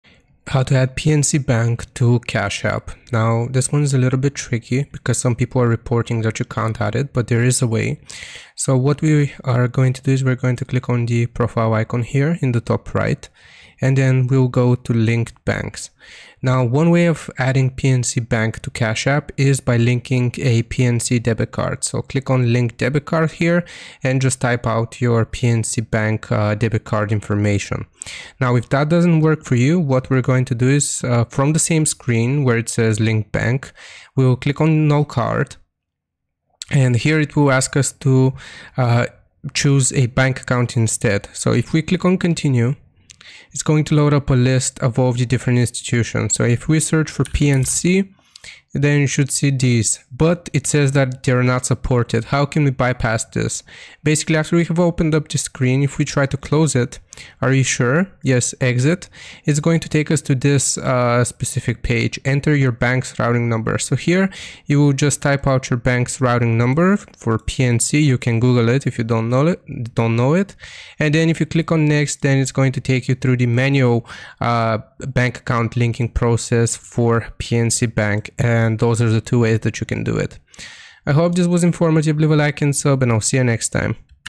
How To Add PNC Bank To Cash App
In this video I show you how to add PNC Bank to Cash App. If you want to learn how to connect PNC to Cash App then this is the tutorial for you.